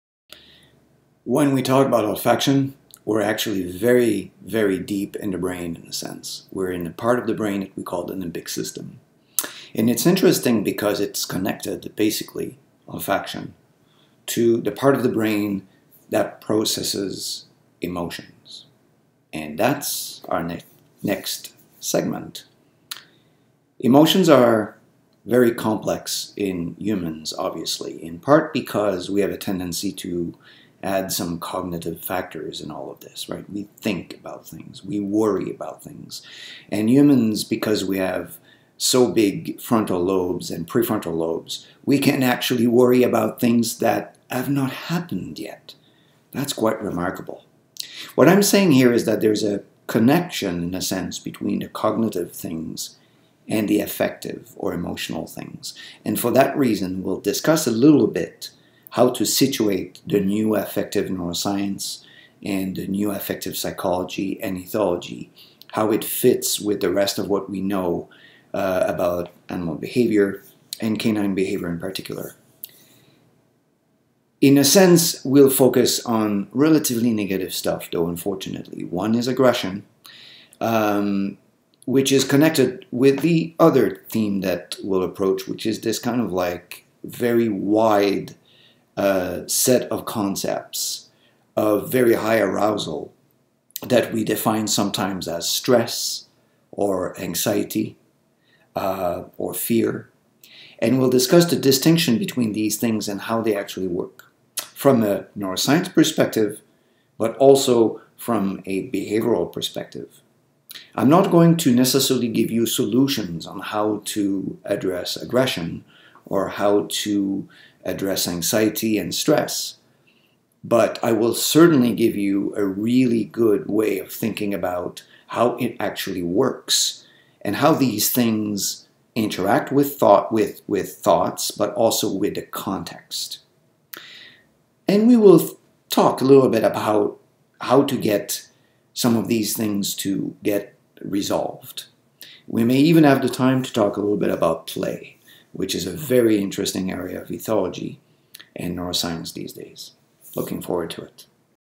Simon Gadbois - Emocje u psów
Dr. Simon Gadbois- etolog i neurobiolog z Dalhousie University w Kanadzie - przybliża nam o czym będzie opowiadał drugiego dnia swojego seminarium w Warszawie (11-13 maja 2018 r.). Szczegóły wydarzenia znajdziecie Państwo na fb